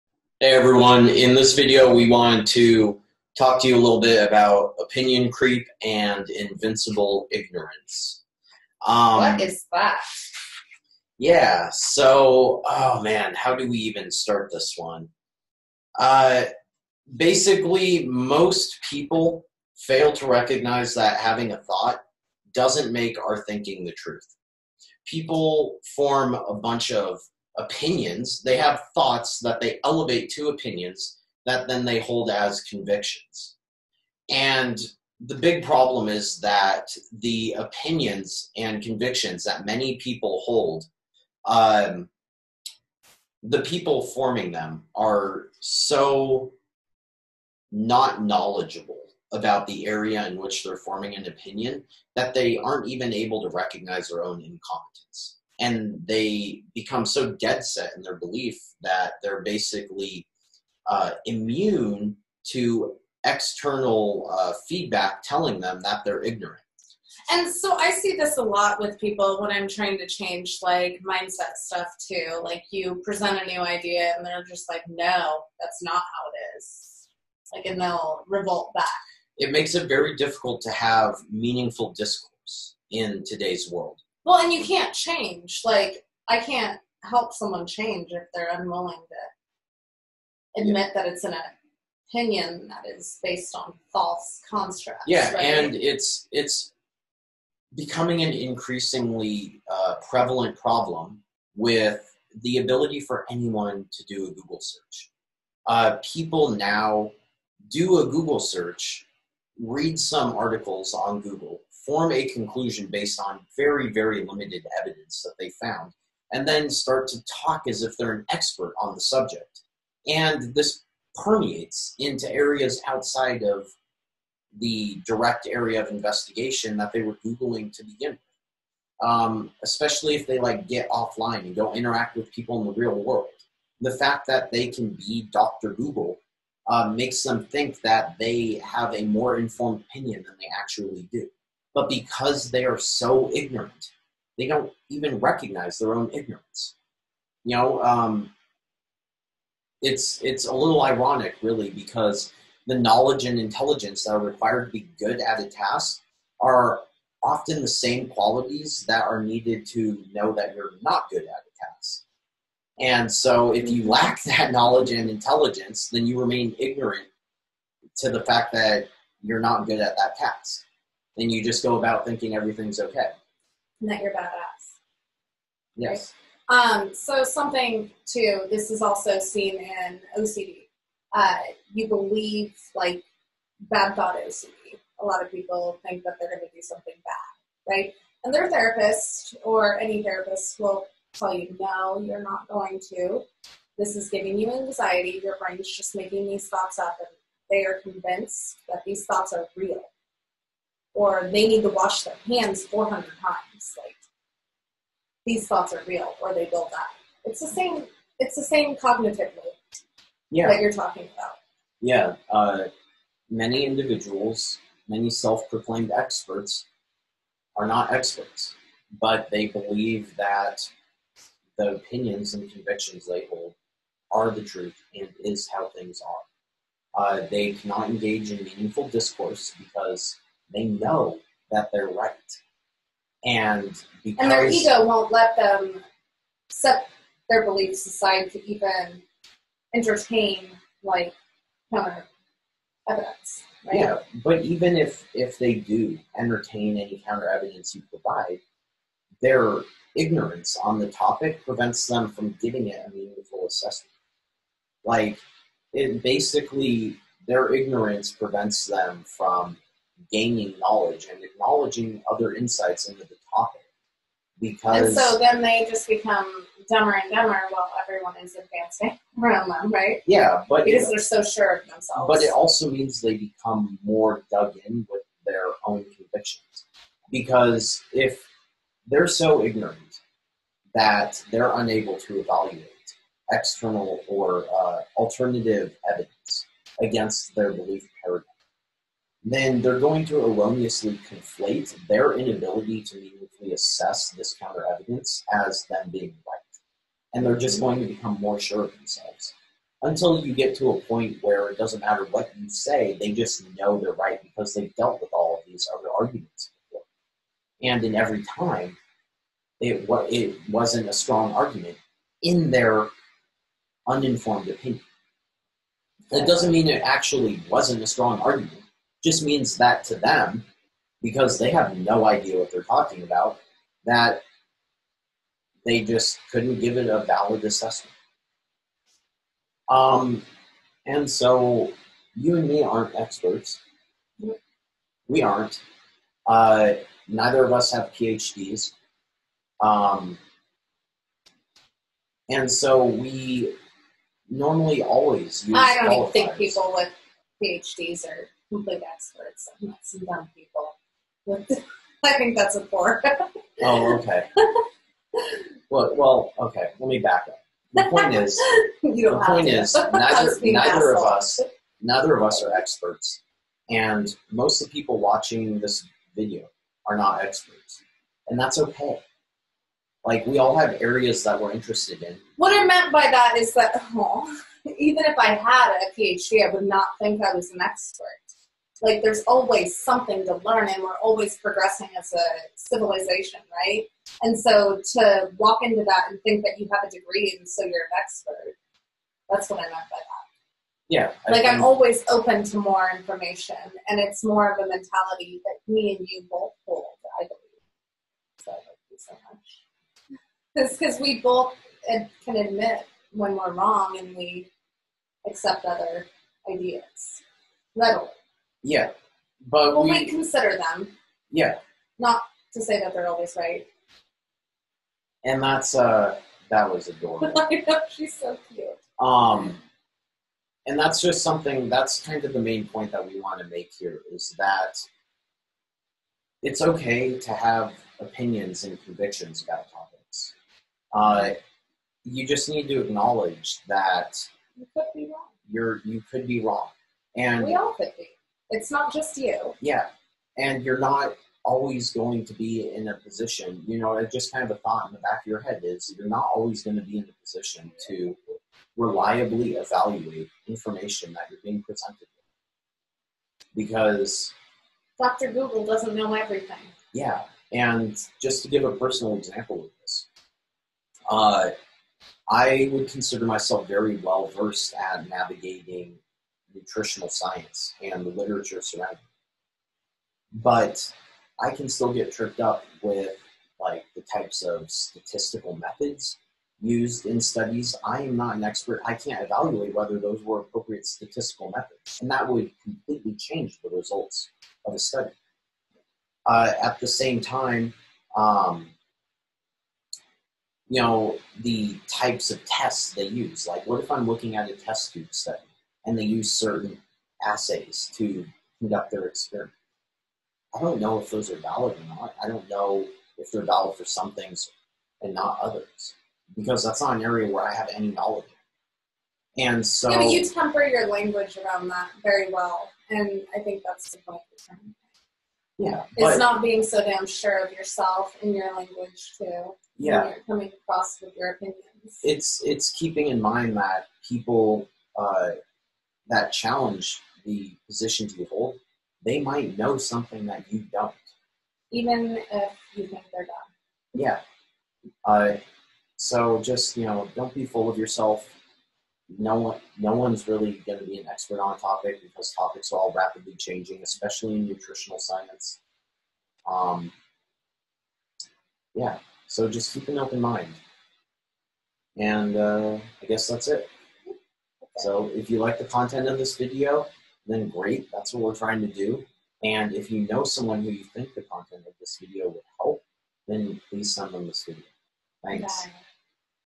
You aren't the expert you think you are
Everyone has 𝘰𝘱𝘪𝘯𝘪𝘰𝘯𝘴. Some opinions are more informed than others. Most people don’t recognize this.⁣
We live in an age where anyone can hop on Google and find and answer to their question… You know, the infamous 𝘥𝘰𝘤𝘵𝘰𝘳 𝘎𝘰𝘰𝘨𝘭𝘦. This creates a sense of knowledge that doesn’t actually exist, and people fail to recognize that having a thought doesn’t make their thinking the truth.⁣
In general, most people don’t appreciate how little they know — the tiniest bit of knowledge makes them feel like experts and talk with an 𝗶𝗹𝗹𝘂𝘀𝗶𝗼𝗻 𝗼𝗳 𝗮𝘂𝘁𝗵𝗼𝗿𝗶𝘁𝘆. The problem is that they are in fact so ignorant on these topics, that they fail to recognize their ignorance and fail to respond to environmental feedback that they are ignorant. ⁣
It's essentially a 𝗗𝘂𝗻𝗻𝗶𝗻𝗴-𝗞𝗿𝘂𝗴𝗲𝗿 𝗲𝗳𝗳𝗲𝗰𝘁 because the knowledge and intelligence that are required to be good at a task are often the same qualities needed to recognize that one is not good at that task. If you lack such knowledge and intelligence, your remain ignorant to your short-comings. ⁣
It’s a fucked situation really, and essentially the reason I refuse to engage with anyone displaying this type of behavior. I just don’t have the time, or the incentive to undo their steadfast beliefs. ⁣
𝗜𝗻 𝘀𝗵𝗼𝗿𝘁, 𝗶𝗻𝗰𝗼𝗺𝗽𝗲𝘁𝗲𝗻𝗰𝗲 𝗿𝗼𝗯𝘀 𝘂𝘀 𝗼𝗳 𝘁𝗵𝗲 𝗮𝗯𝗶𝗹𝗶𝘁𝘆 𝘁𝗼 𝗿𝗲𝗮𝗹𝗶𝘇𝗲 𝗶𝘁, 𝗮𝗻𝗱 𝘄𝗲 𝗮𝗿𝗲 𝗹𝗲𝗳𝘁 𝘄𝗶𝘁𝗵 𝘁𝗵𝗲 𝗲𝗿𝗿𝗼𝗻𝗲𝗼𝘂𝘀 𝗶𝗺𝗽𝗿𝗲𝘀𝘀𝗶𝗼𝗻 𝘄𝗲 𝗮𝗿𝗲 𝗱𝗼𝗶𝗻𝗴 𝗷𝘂𝘀𝘁 𝗳𝗶𝗻𝗲.